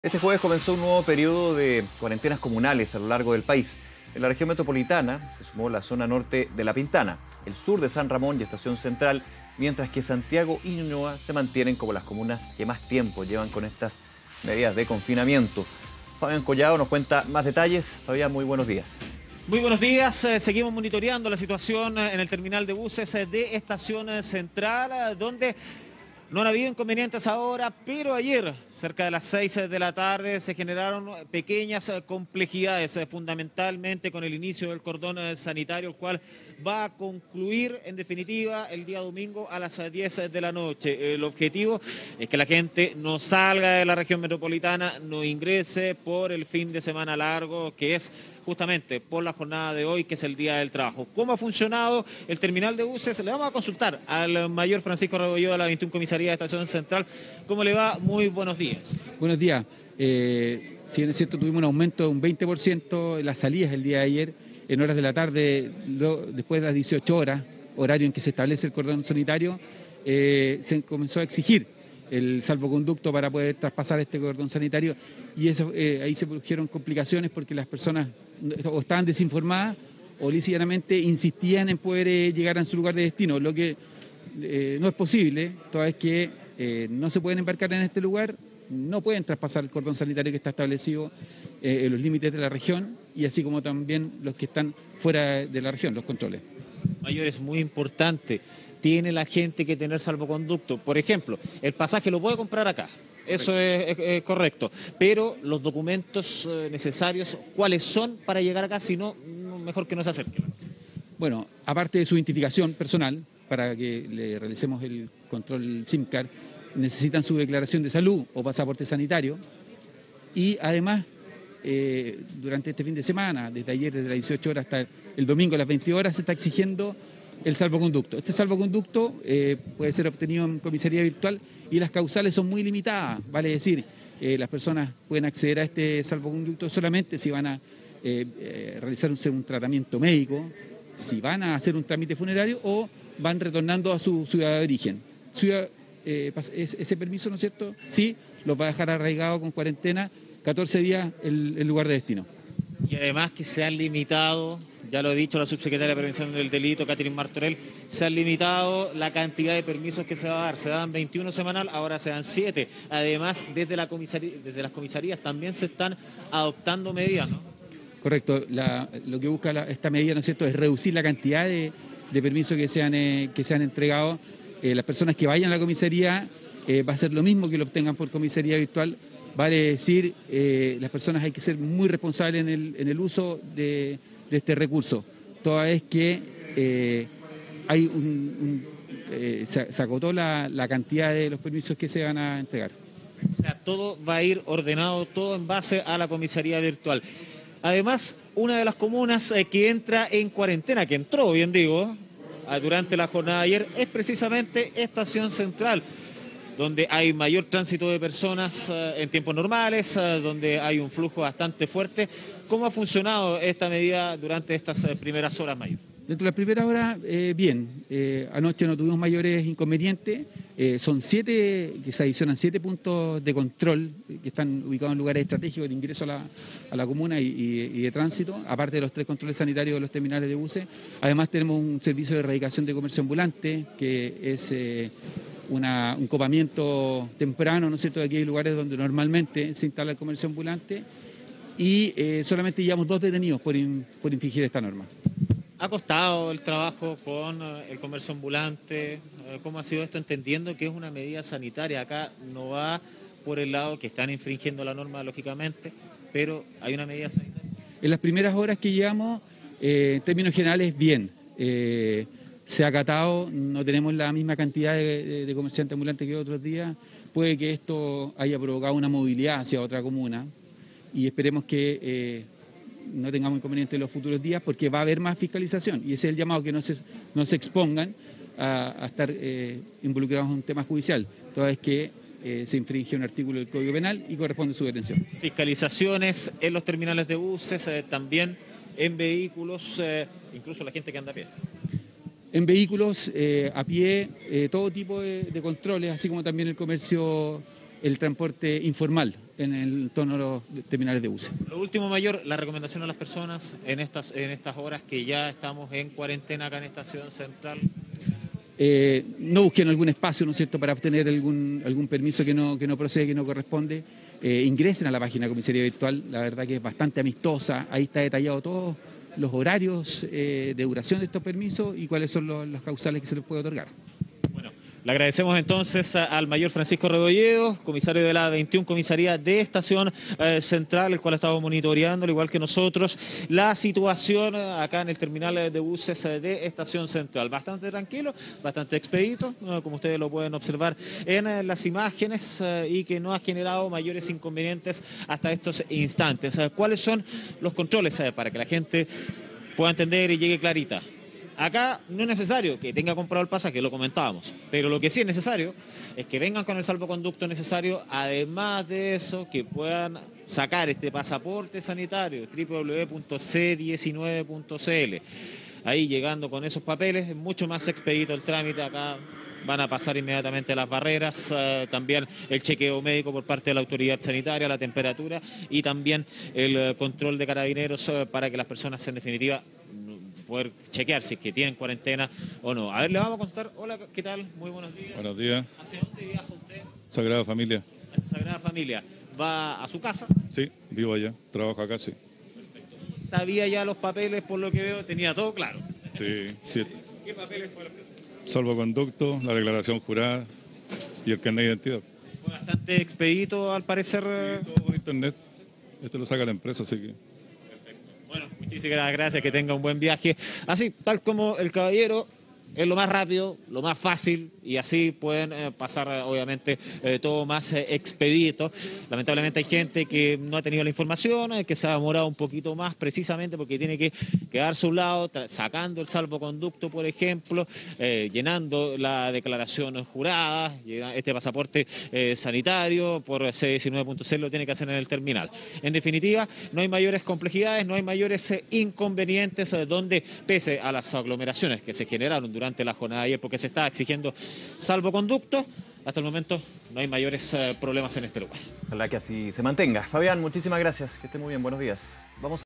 Flujo expedito de personas en Estación Central en inicio de fin de semana largo
Luego de algunas aglomeraciones en terminales durante la jornada de este jueves, se vio menos gente durante la mañana del viernes, con la vigencia de la limitación de permisos semanales impulsada por el Gobierno.